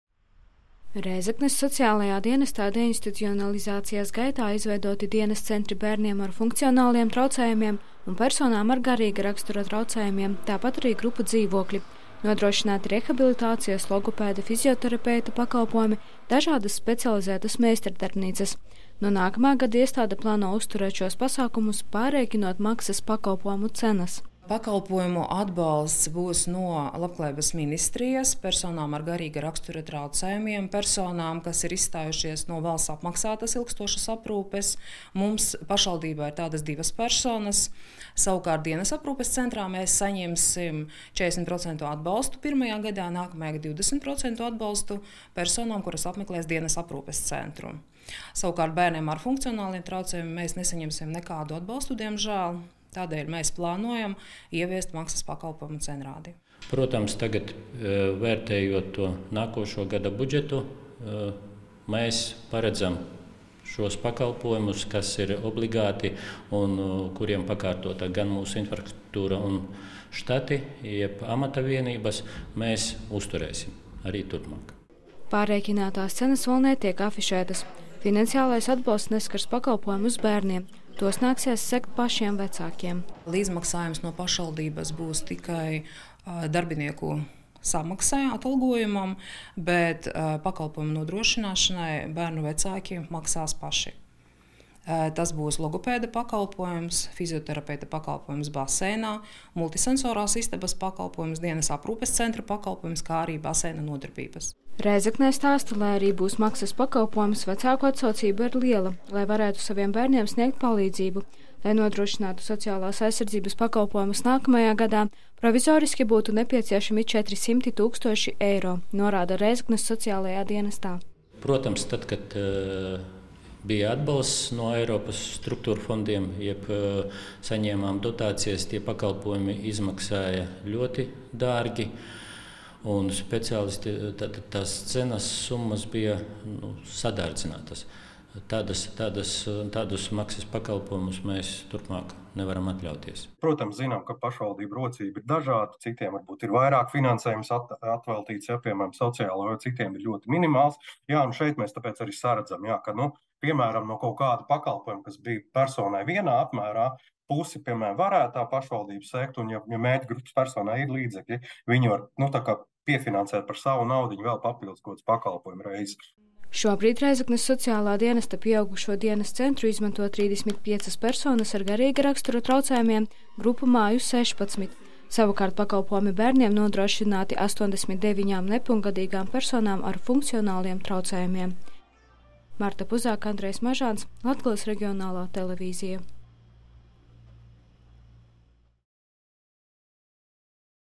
Rēzeknē plāno turpināt deinstitucionalizācijas projektus
Lai arī no nākamā gada deinstitucionalizācijas projektiem Eiropas struktūrfondu finansējuma vairs nebūs, ir atrasts veids, kā šie projekti turpināsies. Daļu finansējuma sagādās valsts, daļu vietvaras un daļa izmaksu gulsies uz nevalstiskajām organizācijām. Tas nozīmē, ka arī Rēzeknē visi iesāktie projekti darbu nepārtrauks.